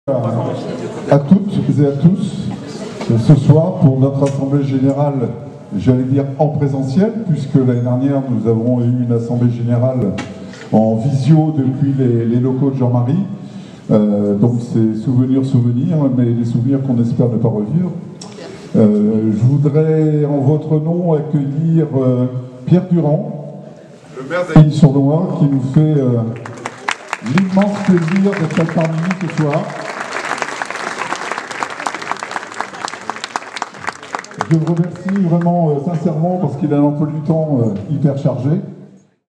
Chaussoy-Épagny : la présence du maire d'Ailly-sur-Noye saluée lors de l'AG du Souffle de la Terre
Dans la soirée du vendredi 4 mars, se tenait l’assemblée générale de l’association Les Spectacles d’Ailly-sur-Noye, organisatrice du Souffle de la Terre. À cette occasion, le président du son et lumière, Christian de Caffarelli, a salué la présence de Pierre Durand dans l’assemblée. Il n’a pas manqué de rappeler au Bonhomme Picard « les relations compliquées » qui existaient auparavant entre l’ancienne maire d’Ailly-sur-Noye, Marie-Hélène Marcel, et son association.